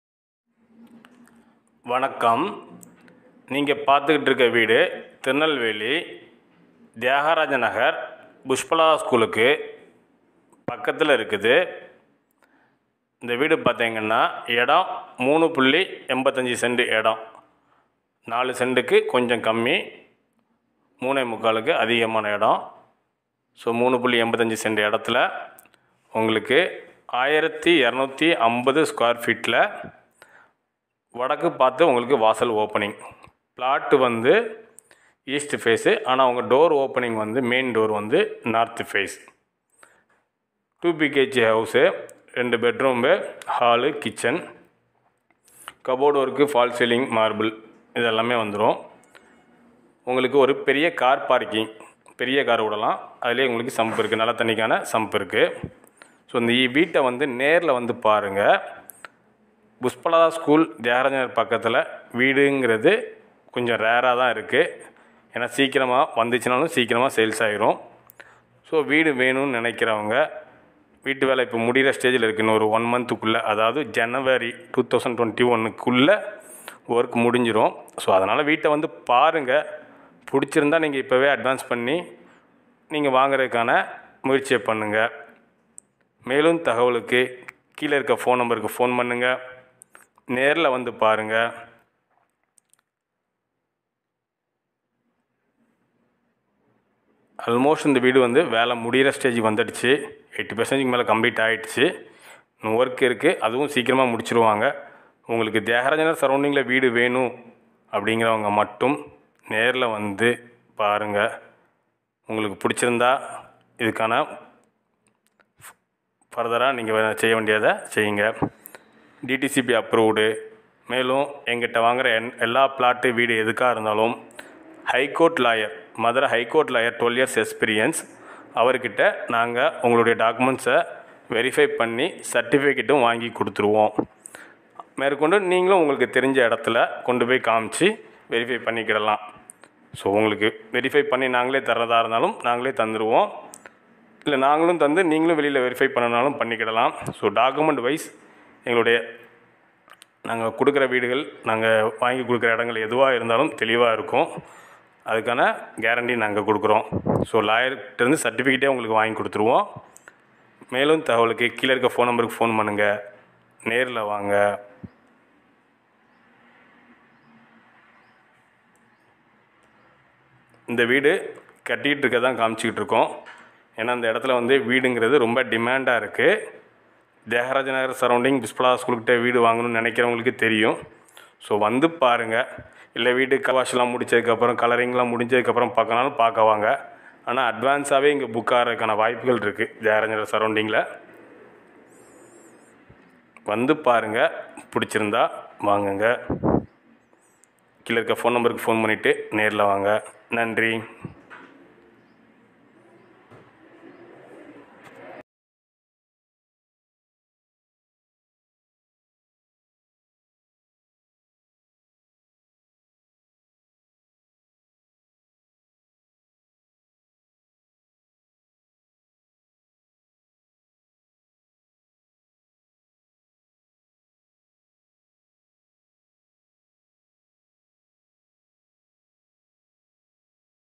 House sale in Near puspalatha school Thiyarajanagar Tirunelveli 70 LAKHS
SARGURU REAL ESTATE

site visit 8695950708

details

3.85 cent

70 lakh

1250 sqft

cupboard work
fall ceiling

car parking
North face door open

west face plot

2bhk

சுப நிகழ்ச்சிகளுக்கு வாழை மரம் கட்டுவதற்கு கிடைக்கும்